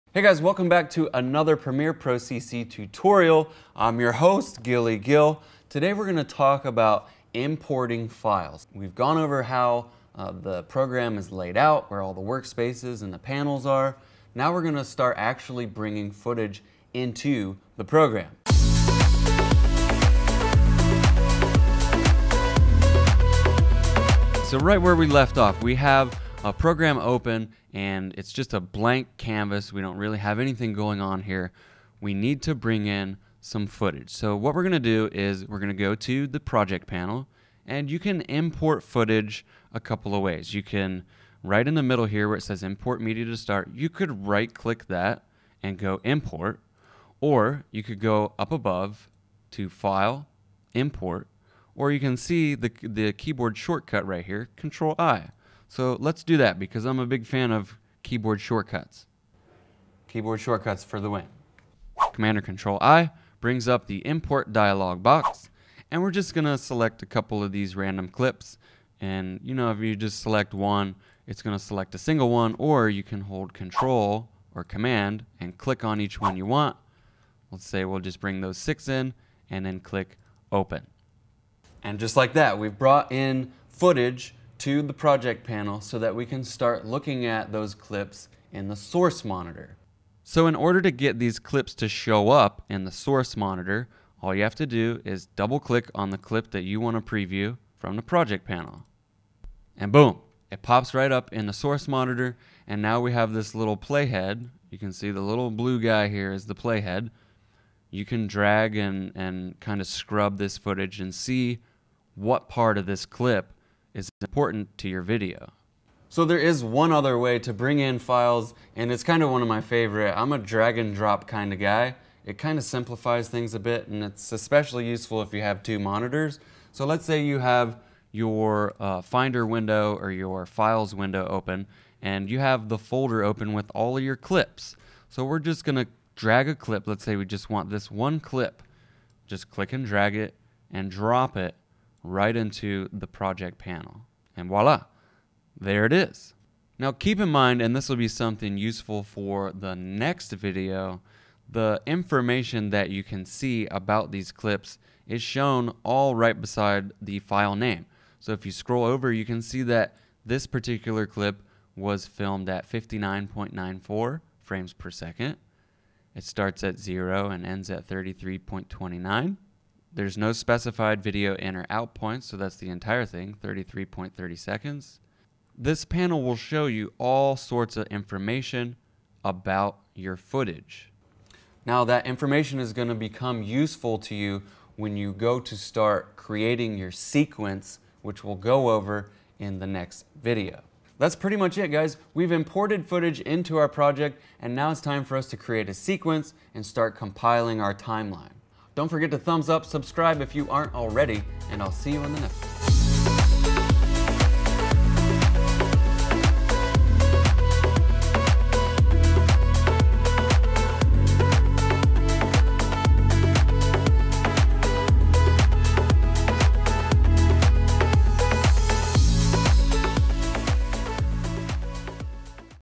Premiere Pro CC Tutorial Series - Importing Files
In this video i'll go over how to properly import your video files to get started editing. Complete with keyboard shortcuts and my personal favorite method of importing footage :)